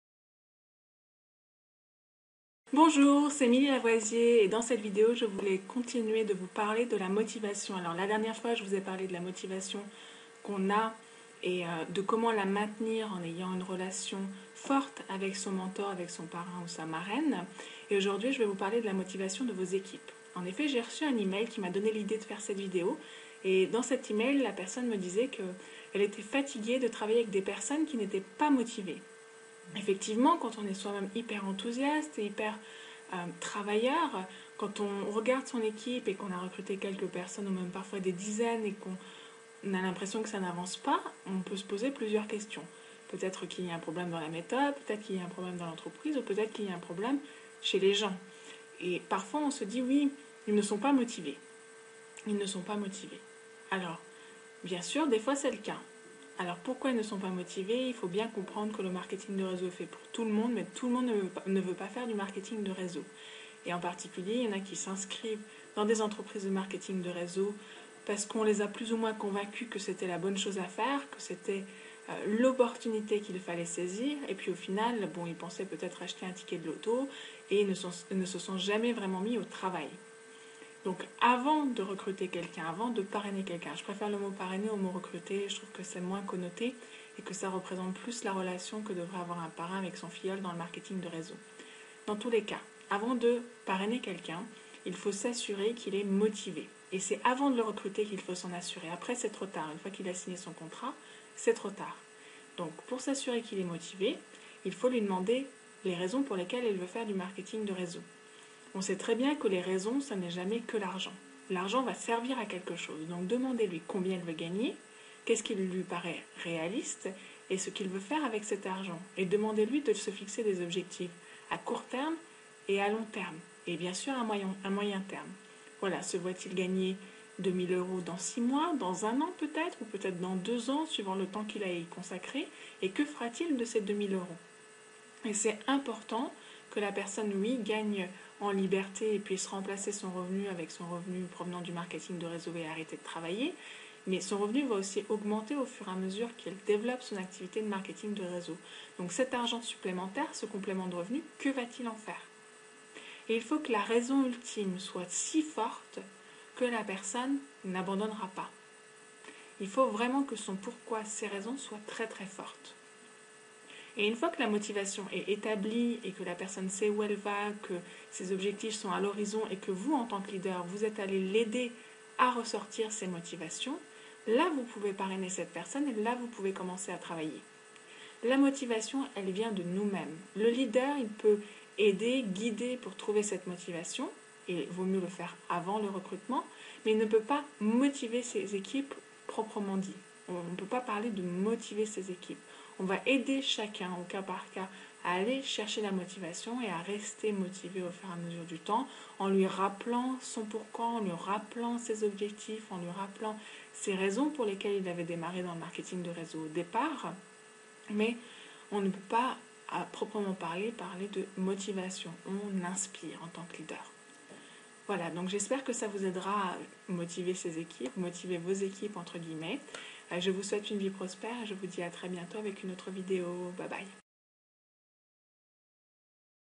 MLM - Avant De Recruter Quelqu'un Dans Votre Marketing De Réseau, Sondez Sa Motivation !
BLOG INSCRIVEZ-VOUS A LA NEWSLETTER

Pour continuer la série de vidéos que Millie Lavoisier a tournées sur le thème de la motivation, elle revient avec cette vidéo qui fait suite à une question qui lui a été posée déjà plusieurs fois. Cette question, elle apparaît chez les leaders qui ont déjà construit un petit groupe d'une vingtaine ou d'un quarantaine de personnes. Et ces leaders trouvent que ça n'avance pas assez vite !

Question : "Millie, je suis fatigué de travailler avec des personnes qui ne sont pas motivées !"

Evidemment, si ce leader est arrivé là où il en est aujourd'hui, c'est parce que lui-même était très motivé, enthousiaste et travailleur.

Ce qui se traduit par un manque de motivation de vos équipes peut venir de différentes raisons abordées dans la vidéo.

Mais pour éviter cette perte de motivation, il faut comprendre que le marketing de réseau est fait pour tout le mmonde, mais que tout le monde ne veut pas faire du marketing de réseau !

Chaque leader doit aller chercher la motivation de son futur filleul AVANT de le recruter, AVANT de le parrainer. APRES c'est trop tard. Millie aborde comment un leader peut aller sonder cette motivation chez un prospect, pour aller jusqu'à trouver LA raison qui fera que cette personne n'abandonnera JAMAIS.